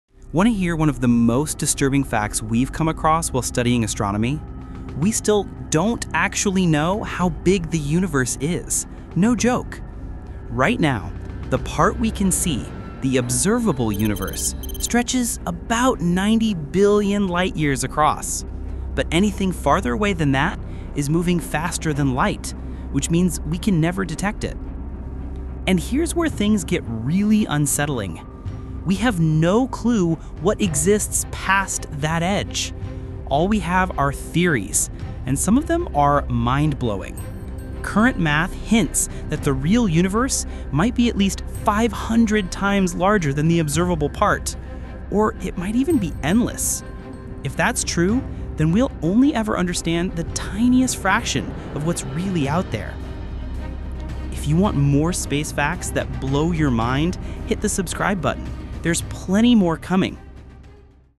The Universe’s Scariest Truth
Want to hear one of the most disturbing facts we've come across while studying astronomy? We still don't actually know how big is the universe. This science explained video dives into these incredible universe facts, detailing how space mysteries persist even with our best tools. We discuss the cosmos and the true universe size, pushing our understanding beyond the cosmic horizon.